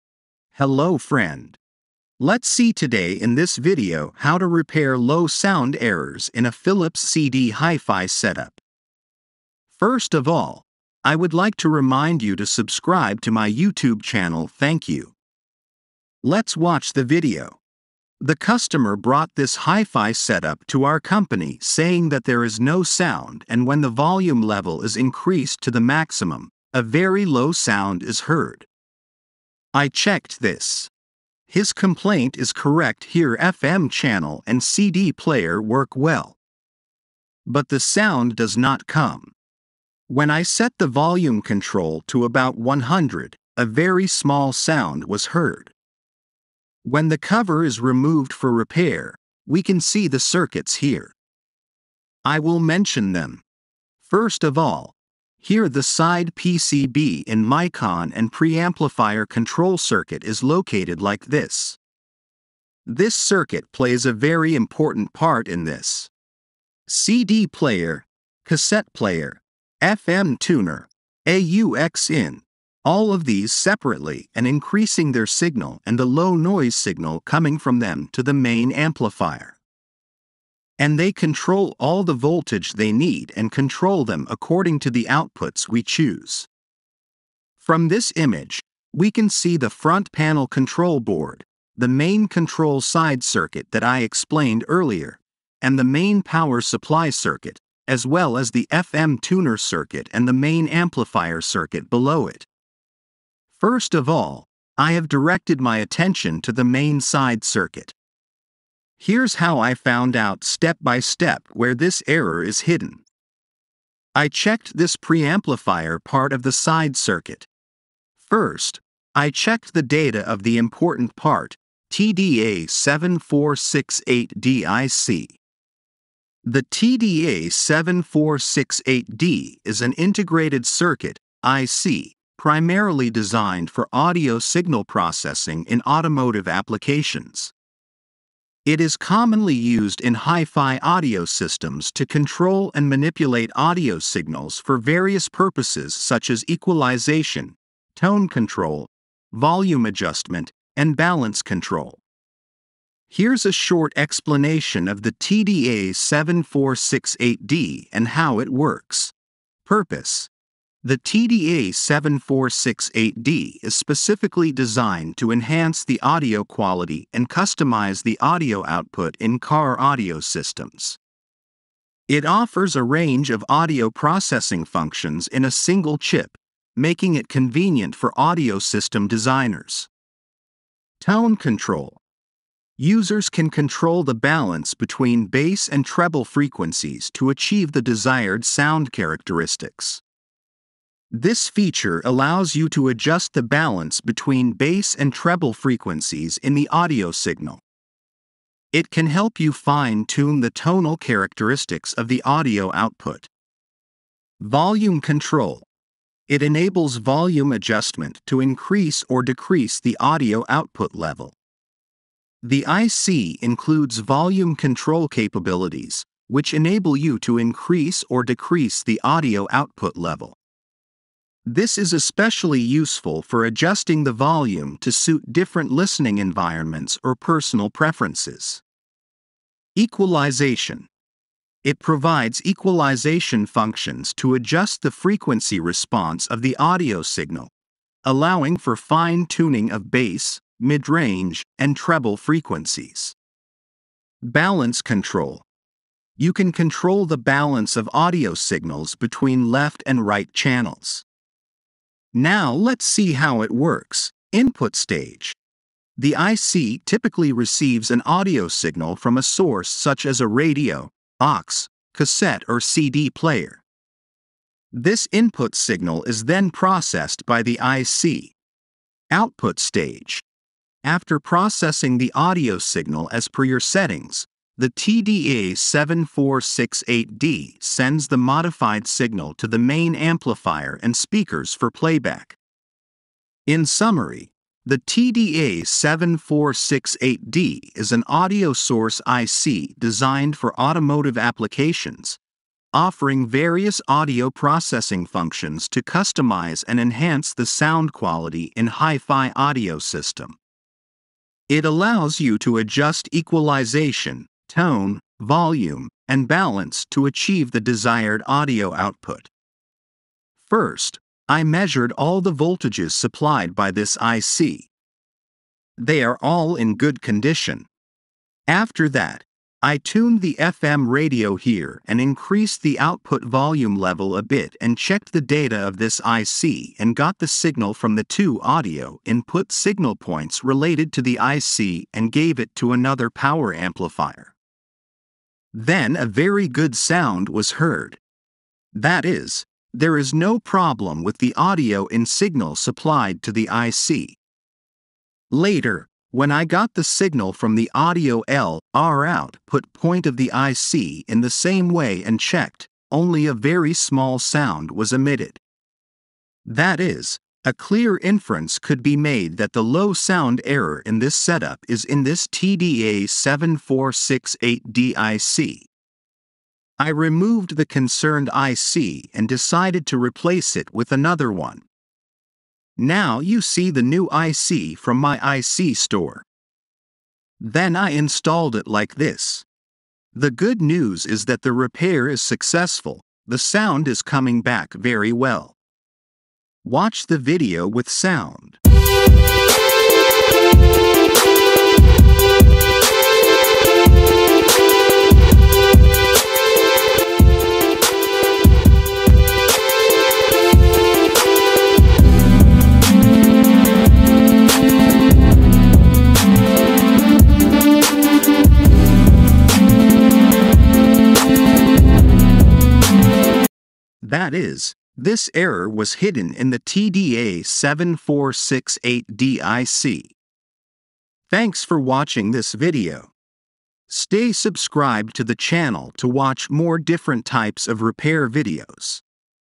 PHILIPS Hi Fi Setup Low Sound Repair | Philips Hi Fi Sound Fault 🔊 | බලමුද..? මෙහෙමයි හැදුවේ..
Hello Friends.. Welcome To The My Repair Channel...

Today in this video how to repair Low Sound errors in a PHILIPS CD Hi fi  Setup.

"PHILIPS Hi-Fi Setup Low Sound Repair | Fault Diagnosis: TDA7468D IC"

🔊 Is your PHILIPS Hi-Fi Setup delivering low sound or poor audio quality? In this comprehensive video, we'll guide you through the troubleshooting and repair process to get your Hi-Fi system back to its full sonic glory!

🔧 Our expert technician will walk you through each step of the repair, with a special focus on identifying the culprit: the TDA7468D IC (Integrated Circuit). Discover how this tiny but critical component can affect your audio output and how to replace it if necessary.

🔍 Here's what you can expect in this video:

1️⃣ Introduction to the TDA7468D IC and its role in your PHILIPS Hi-Fi Setup.
2️⃣ Symptoms of low sound or audio issues that may be linked to the TDA7468D IC.
3️⃣ A step-by-step guide on diagnosing the fault and pinpointing the IC's potential malfunction.
4️⃣ Detailed instructions on safely disassembling your Hi-Fi system for inspection and repair.
5️⃣ Tips on sourcing a replacement TDA7468D IC if needed.
6️⃣ A live demonstration of the IC replacement process, including soldering techniques.
7️⃣ Reassembly and testing to ensure your Hi-Fi Setup is working flawlessly.

🔊 With our easy-to-follow instructions, you'll be able to troubleshoot and fix the low sound issue in your PHILIPS Hi-Fi Setup like a pro. Say goodbye to muffled audio and rediscover the joy of your music collection.

Get ready to revive your Hi-Fi system's sound quality with this informative repair tutorial!